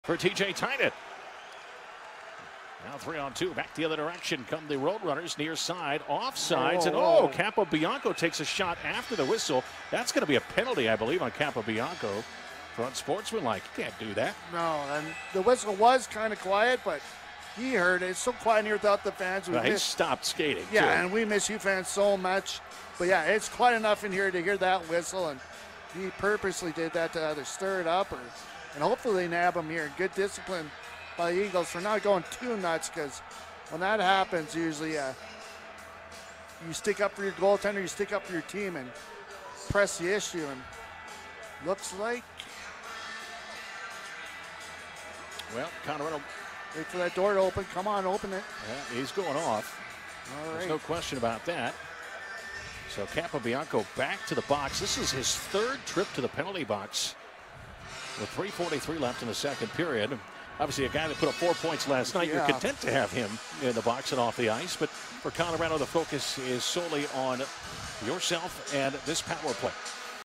Kyle Capobianco Unsportsmanlike Conduct Penalty For Shooting Puck After Whistle
Tucson Roadrunners @ Colorado Eagles 02/27/21